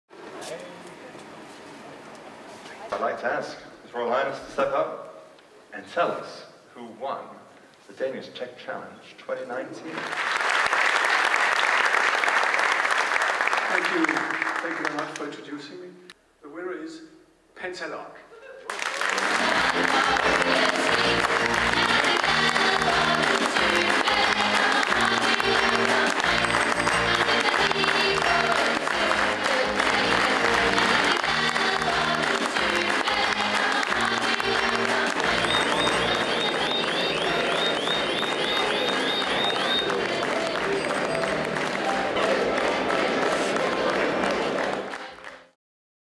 Kronprins Frederik spredte glæde til prisfest
Efter knap to uger i Schweiz spredte en veloplagt kronprins Frederik igen glæde omkring sig på et kongeligt job i København.

Med et stort smil på læben ankom kronprins Frederik til uddelingen af Industriens Fonds Iværksætterpris ved Danish Tech Challenge Award Show i The Plant på Amager.

Det var sjette gang, at kronprinsen var med til at hædre vinderen af Danmarks største hardwarekonkurrence og det var værdsat.

Oppe på scenen storroste han årets vinderteam fra firmaet Pentalock, der har udviklet en intelligent cykellås, og overrakte dem derefter den tilhørende check på hele 500.000 kroner.